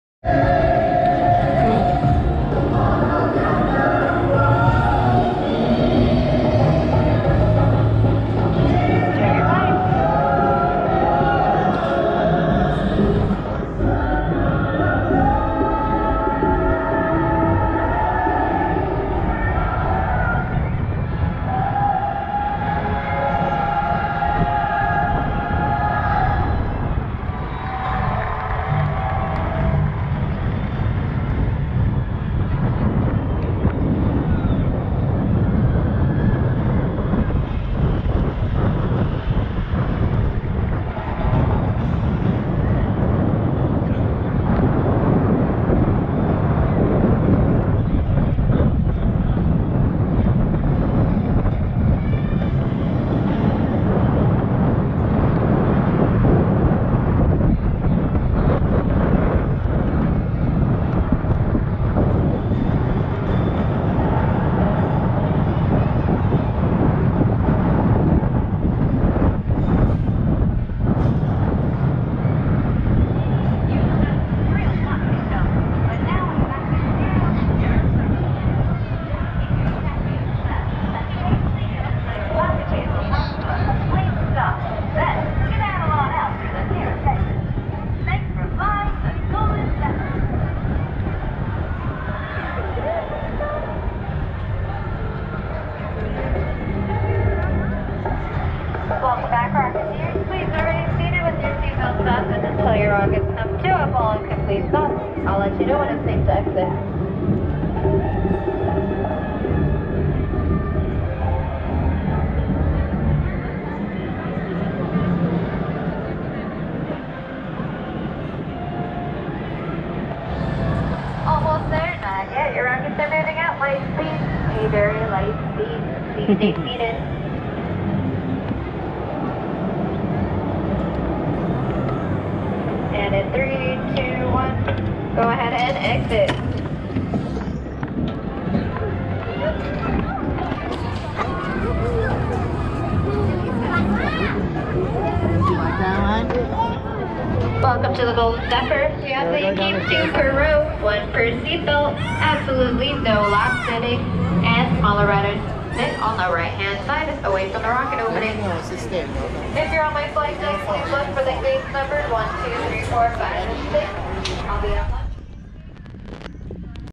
Golden Zephyr POV 4K Video - Night
Disney California Adventure Golden Zephyr Point of View 4K Video - Night